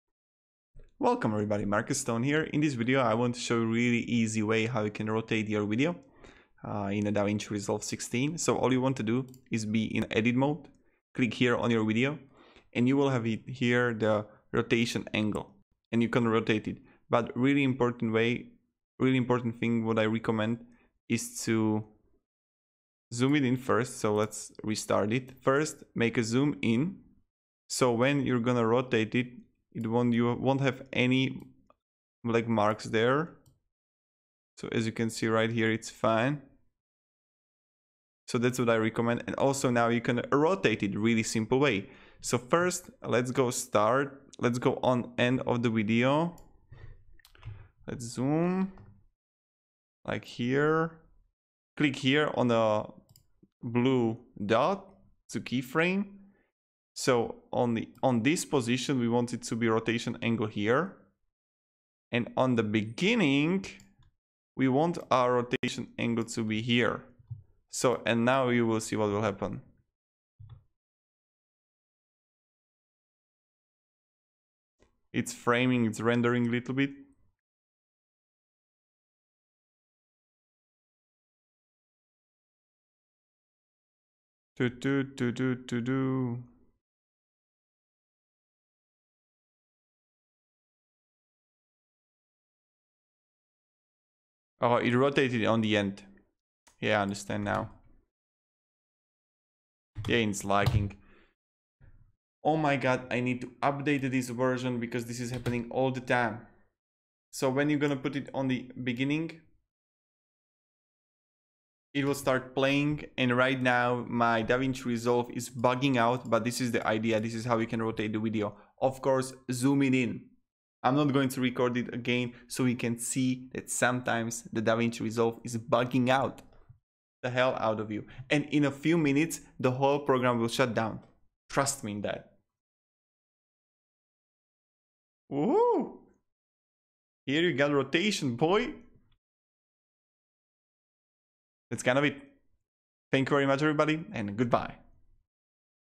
How To Rotate Video in Da vinci Resolve 16
‏‏‎ ‎ In This video I will show you simple way how you can rotate video in Da vinci resolve 16 :) , transferwise ‏‏‎ ‎

GET AMAZING FREE Tools For Your Youtube Channel To Get More Views:
Tubebuddy (For GROWTH on Youtube):